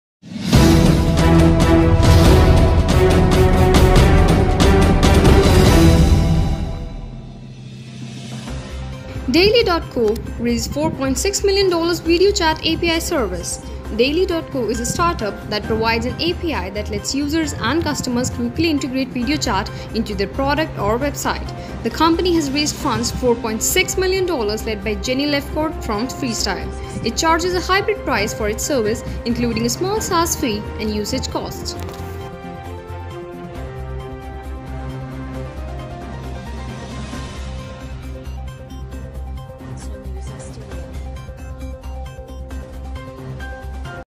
Daily.co raises $4.6M video chat API service
Daily.co is a startup that provides an API that lets users and customers quickly integrate video chat into their product or website. The company has raised funds $4.6 million led by Jenny Lefcourt from Freestyle. It charges a hybrid price for its service, including a small SaaS fee and usage costs.